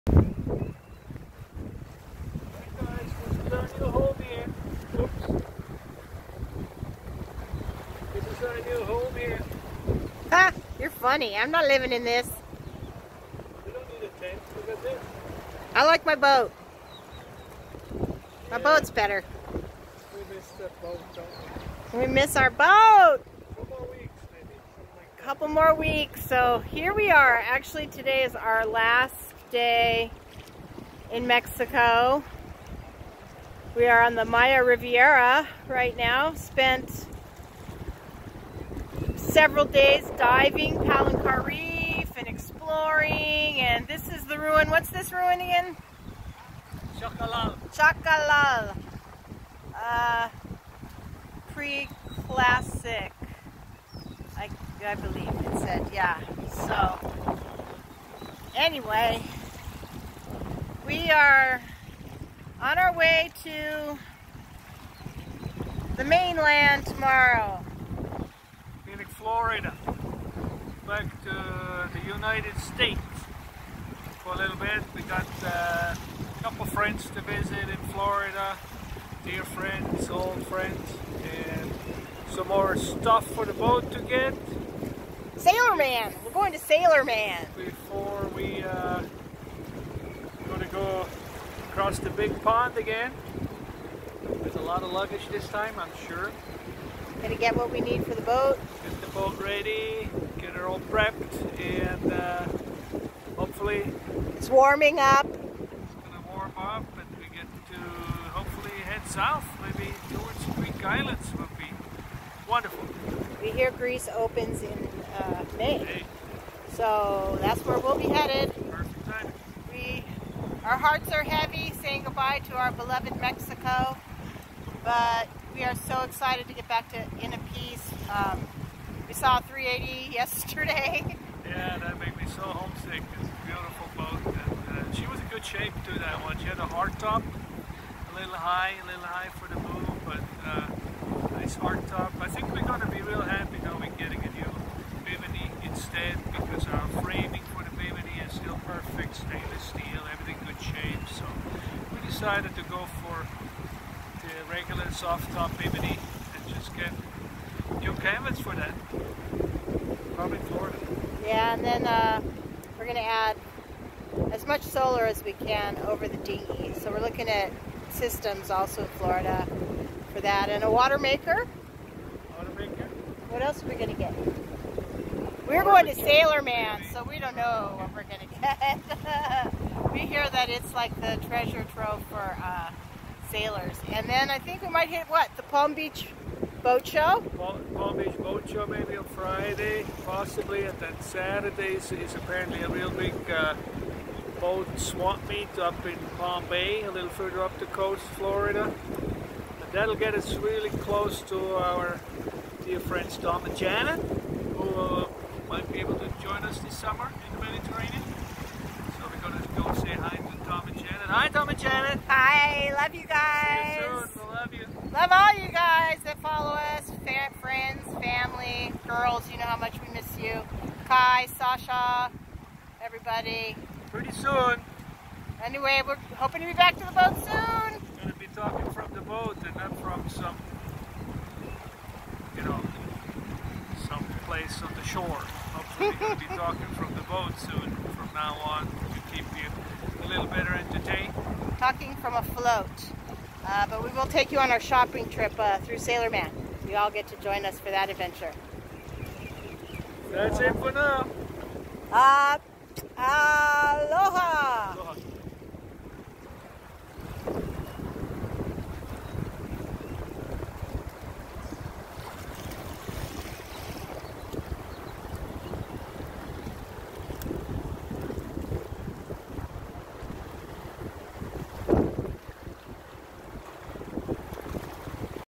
Our last night in Mexico. Puerto Aventura, Quintana Roo
It is with heavy hearts we say good bye to our beloved Mexico. She has been good to us during our winter stay here. Fun with family, friends, good food and drink. Now we are off to Florida to buy some needful things for INNA PEACE as we work our way back to our adorable Lagoon 380 in Montenegro and complete her refit. Greece opens up in May so if you would like to join us in Greece this summer, please drop us a message at
Let us know when you would like to jump on board and we can work out the details.
Happy Sailing!!